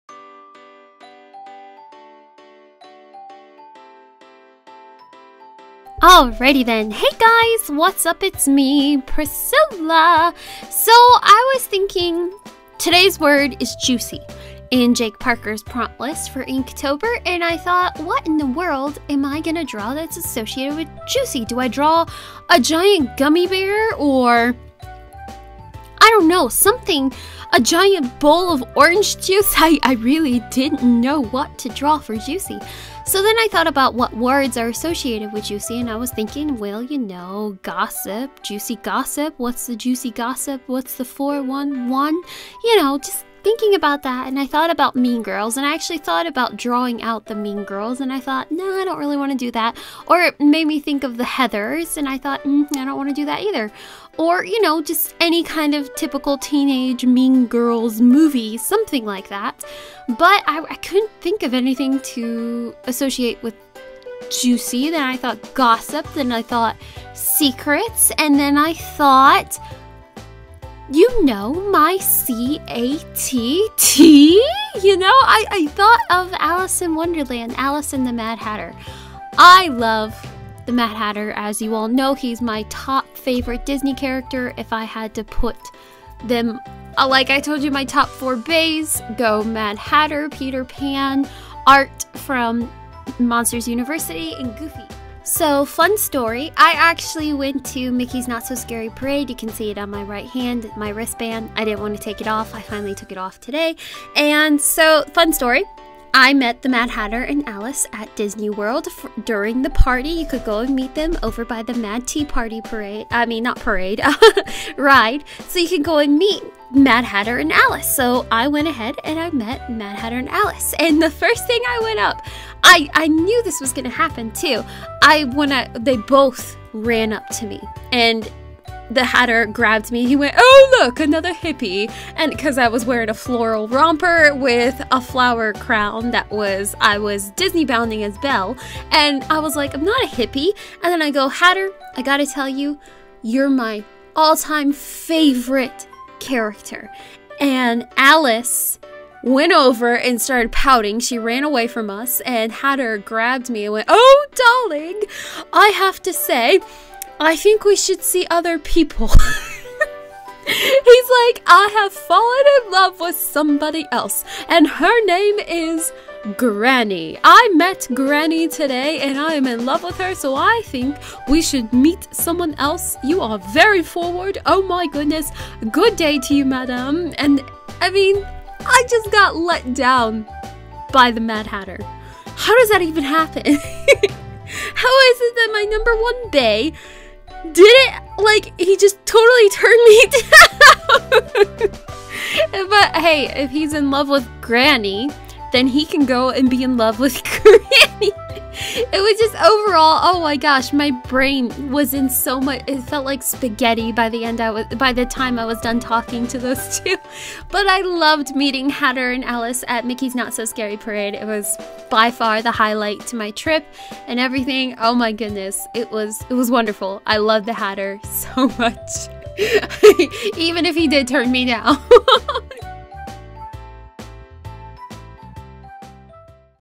Alice and Hatter "JUICY" Day 23 of INKTOBER 2017
It's funny how the mind travels. For today's Inktober I drew Disney's Alice and Mad Hatter from Alice in Wonderland!